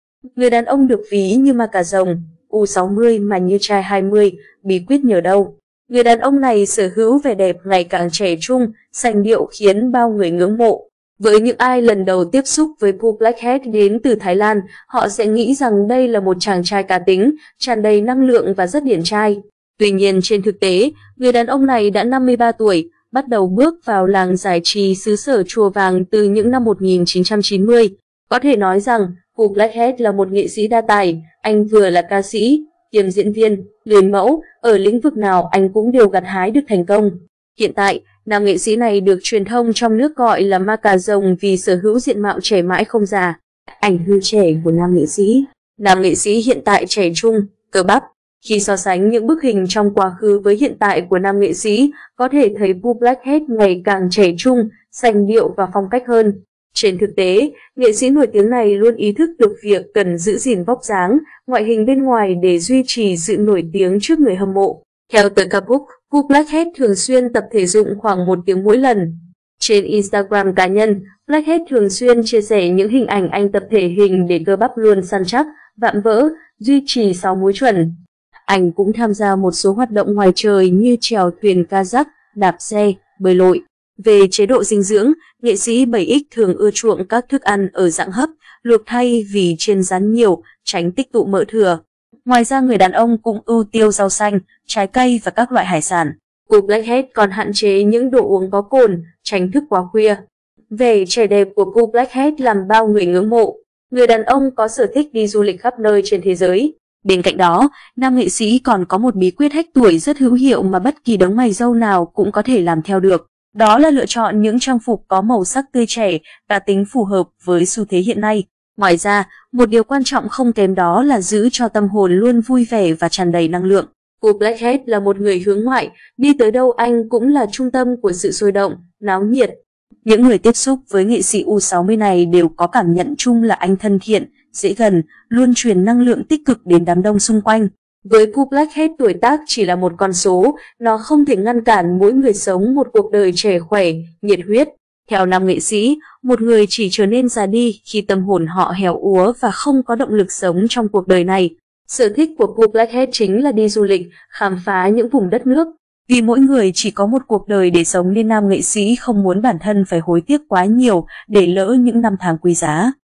Người đàn ông được ví như 'ma cà rồng', U60 mà như trai 20, bí quyết nhờ đâu?
Người đàn ông này sở hữu vẻ đẹp ngày càng trẻ trung, sành điệu khiến bao người ngưỡng mộ.Pu Blackhead là một người hướng ngoại, đi tới đâu anh cũng là trung tâm của sự sôi động, náo nhiệt. Những người tiếp xúc với nghệ sĩ U60 này đều có cảm nhận chung là anh thân thiện, dễ gần, luôn truyền năng lượng tích cực đến đám đông xung quanh. Với Pu Blackhead tuổi tác chỉ là một con số, nó không thể ngăn cản mỗi người sống một cuộc đời trẻ khỏe, nhiệt huyết.
Bên cạnh đó, nam nghệ sĩ còn có một bí quyết “hack tuổi” rất hữu hiệu mà bất kỳ đấng mày râu nào cũng có thể làm theo được đó là lựa chọn những trang phục có màu sắc tươi trẻ, cá tính phù hợp với xu thế hiện nay. Ngoài ra, một điều quan trọng không kém đó là giữ cho tâm hồn luôn vui vẻ và tràn đầy năng lượng.
Có thể nói rằng, Pu Blackhead là một nghệ sĩ đa tài, anh vừa là ca sĩ, kiêm diễn viên, người mẫu, ở lĩnh vực nào anh cũng đều gặt hái được thành công. Hiện tại, nam nghệ sĩ này được truyền thông trong nước gọi là "ma cà rồng" vì sở hữu diện mạo trẻ mãi không già.
Về chế độ dinh dưỡng, nghệ sĩ 7X thường ưa chuộng các thức ăn ở dạng hấp, luộc thay vì chiên rán nhiều, tránh tích tụ mỡ thừa. Ngoài ra người đàn ông cũng ưu tiêu rau xanh, trái cây và các loại hải sản. Pu Blackhead còn hạn chế những đồ uống có cồn, tránh thức quá khuya.
Khi so sánh những bức hình trong quá khứ với hiện tại của nam nghệ sĩ, có thể thấy Pu Blackhead ngày càng trẻ trung, sành điệu và phong cách hơn. Trên thực tế, nghệ sĩ nổi tiếng này luôn ý thức được việc cần giữ gìn vóc dáng, ngoại hình bên ngoài để duy trì sự nổi tiếng trước người hâm mộ.
Nam nghệ sĩ hiện tại trẻ trung, cơ bắp.
Người đàn ông có sở thích đi du lịch khắp nơi trên thế giới.
Ảnh thời trẻ của nam nghệ sĩ.
Theo tờ Kapook, Pu Blackhead thường xuyên tập thể dụng khoảng 1 tiếng mỗi lần. Trên Instagram cá nhân, Blackhead thường xuyên chia sẻ những hình ảnh anh tập thể hình để cơ bắp luôn săn chắc, vạm vỡ, duy trì 6 múi chuẩn. Anh cũng tham gia một số hoạt động ngoài trời như chèo thuyền kayak, đạp xe, bơi lội.
Với những ai lần đầu tiếp xúc với Pu Blackhead, đến từ Thái Lan, họ sẽ nghĩ rằng đây là một chàng trai cá tính, tràn đầy năng lượng và rất điển trai. Tuy nhiên trên thực tế, người đàn ông này đã 53 tuổi, bắt đầu bước vào làng giải trí xứ sở chùa Vàng từ những năm 1990.
Vẻ trẻ đẹp của Pu Blackhead làm bao người ngưỡng mộ.
Theo nam nghệ sĩ, một người chỉ trở nên già đi khi tâm hồn họ héo úa và không có động lực sống trong cuộc đời này. Sở thích của Pu Blackhead chính là đi du lịch, khám phá những vùng đất nước. Vì mỗi người chỉ có một cuộc đời để sống nên nam nghệ sĩ không muốn bản thân phải hối tiếc quá nhiều, để lỡ những năm tháng quý giá.